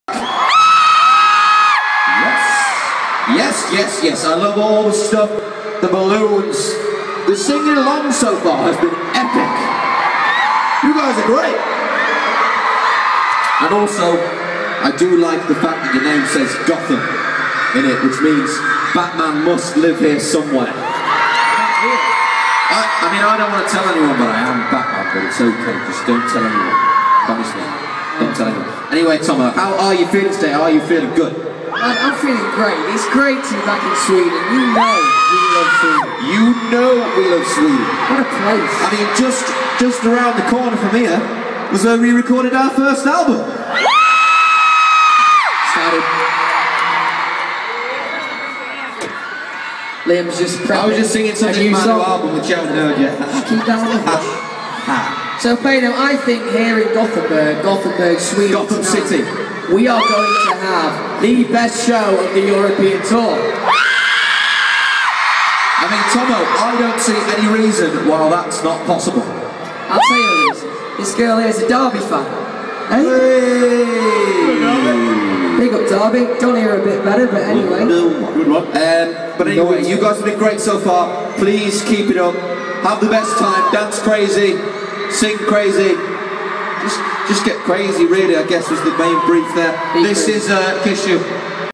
One Direction- Liam & Louis Talking- On The Road Again Tour- Gothenburg, Sweden, 23/6-2015
BEST NIGHT OF MY LIFE EVER. Watch in HD!
I know that the recording is shit but I hope you like it anyway! :)
And excuse my ugly singing/screaming